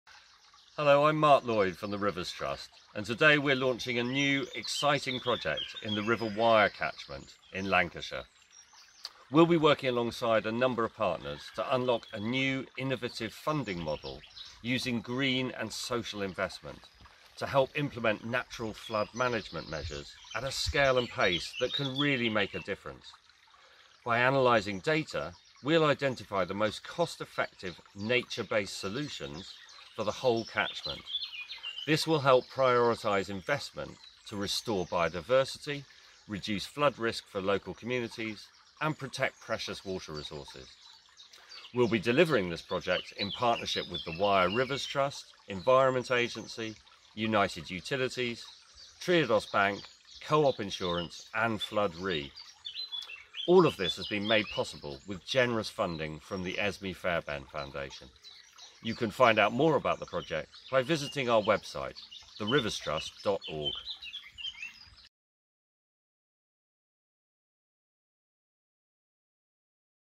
Mark Lloyd introduces new project in the Wyre catchment
The Rivers Trust and Wyre Rivers Trust are leading a pioneering project which will investigate innovative funding opportunities for implementing natural flood management measures on the Wyre and its tributaries.

The project will evaluate the effectiveness and certainty of a variety of natural flood management solutions. This will help prioritise investment in solutions which offer the greatest social, economic and environmental return for the catchment and its community.

Why is this project needed?
At a time when we are facing a climate emergency, we must find new ways to invest in recovery of the natural processes that protect and support us, at a scale and pace that can make a difference.

Hard engineering alone will not address our future flood risk challenges and must be supplemented by natural solutions.

Who's involved?
This project is being delivered by The Rivers Trust, Wyre Rivers Trust, Environment Agency, United Utilities, Triodos Bank, Co-op Insurance and Flood Re with funding from Esmée Fairbairn Foundation.